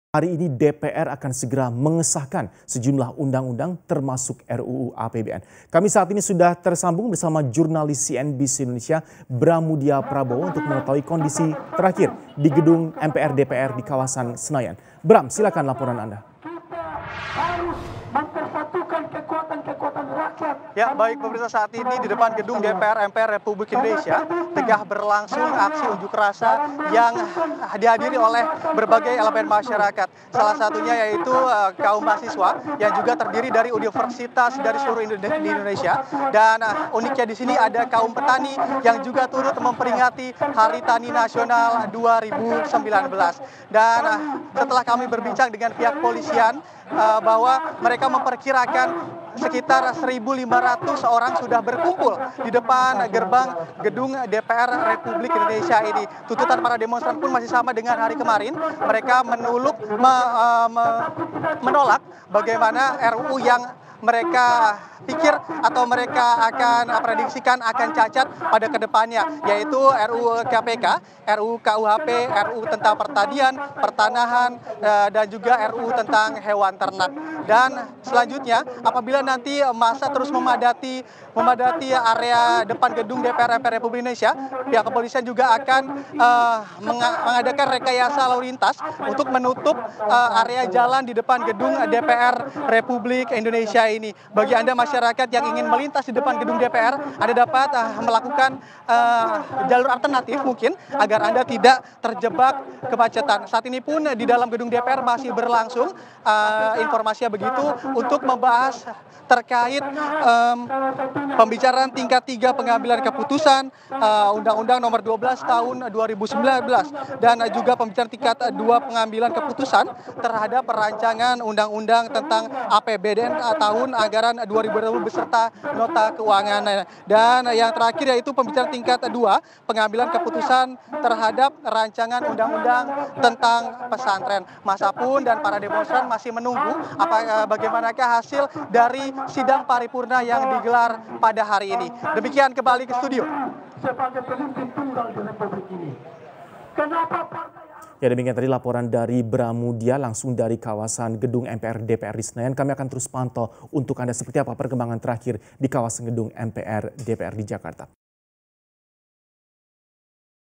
Unjuk Rasa Warnai Sidang Pengesahan Sejumlah RUU
Sidang DPR RI yang akan mengesahkan sejumlah RUU termasuk RUU APBN 2020 diwarnai aksi unjuk rasa sekitar 1500 orang yang berasal dari sejumlah elemen masyarakat, termasuk mahasiswa hingga para petani.

Para demonstran yang  menggelar aksi di depan gedung DPR RI ini menuntut dibatalkannya pengesahan sejumlah RUU yang dinilai cacat seperti RUU KPK, RUU KUHP hingga RUU Sistem Budidaya Pertanian Berkelanjutan.

Selengkapnya saksikan laporan Bramudya Prabowo dalam Squawk Box, CNBC Indonesia (Selasa, 24/9/2019).